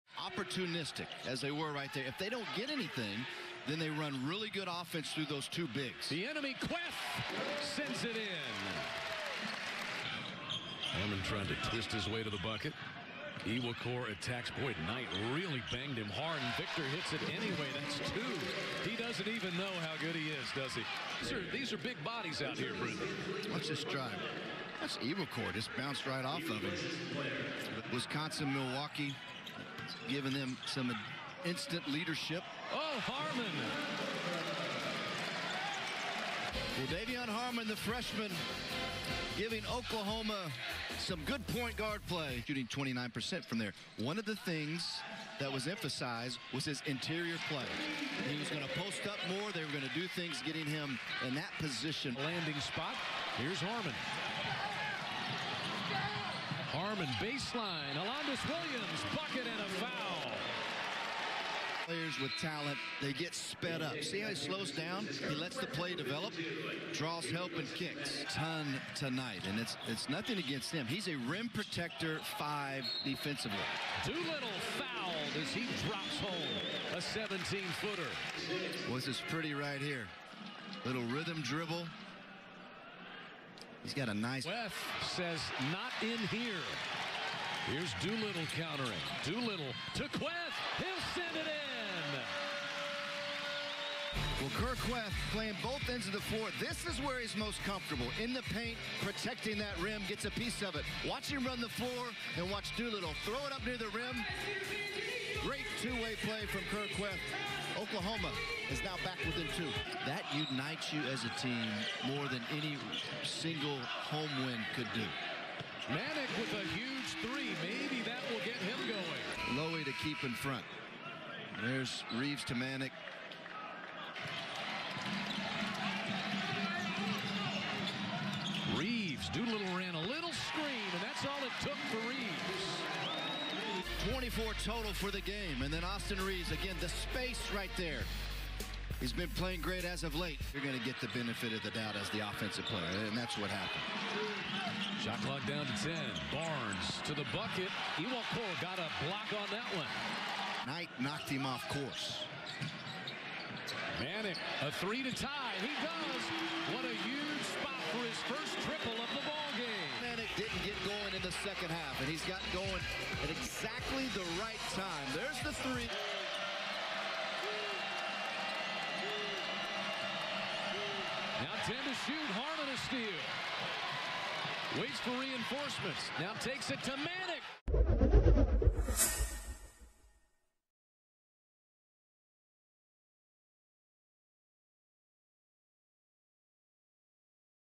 Oklahoma Basketball Top Plays vs. William & Mary (2019-20) | Stadium
Oklahoma got big games from F Kristian Doolittle and G Austin Reaves, who combined for 37 points, as the Sooners came back to beat William & Mary 75-70. Catch their best plays here.